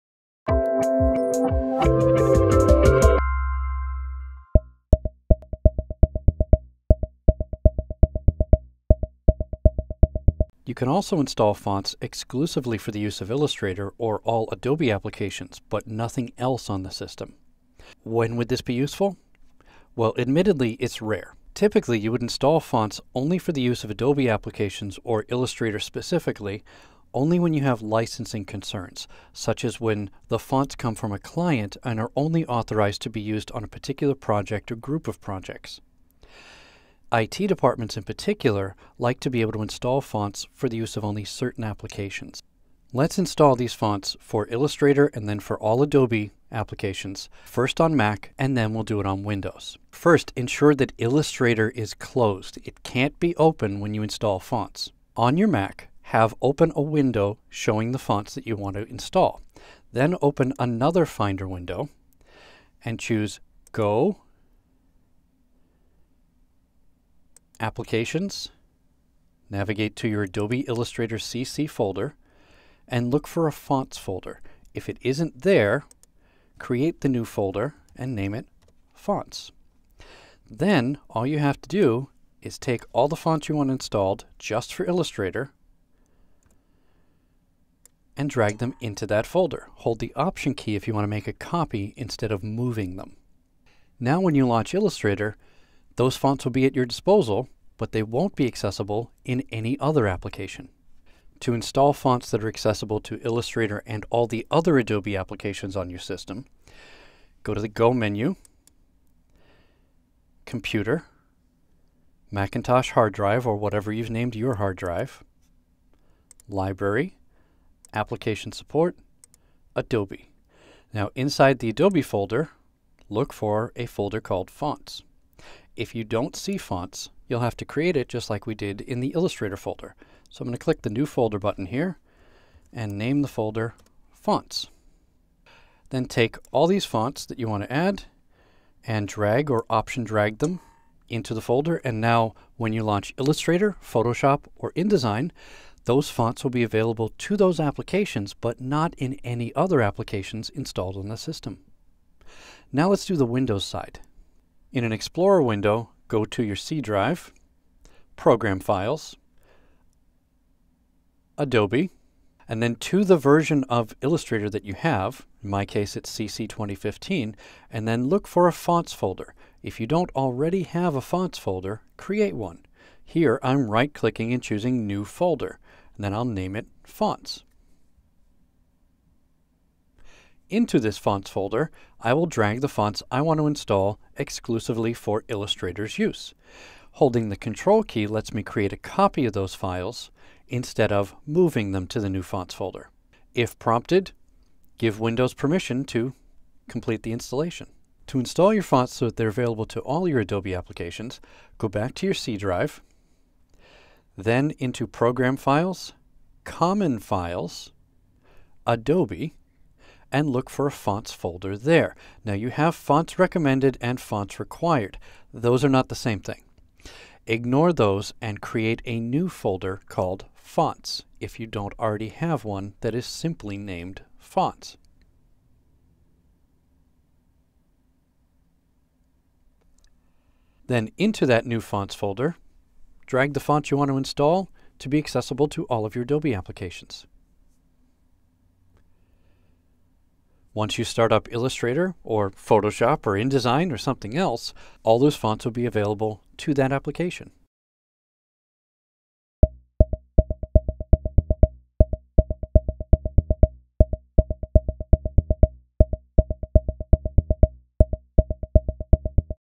Installing Fonts Only for Use by Adobe Illustrator and Other Adobe Applications
For a variety of reasons you, or your I.T. department, might want to install fonts but limit their availability to only one or more Adobe Creative Cloud applications. Fonts would then be available to Photoshop, InDesign, Illustrator, or other Adobe applications, but not available for use in Microsoft Word or any non-Adobe application.

In this tutorial clip you’ll learn how easy it is to install fonts exclusively for the use of one or more Adobe applications on MacOS and Windows.

Excerpted from Pariah Burke's comprehensive Pluralsight video course, Illustrator CC Typography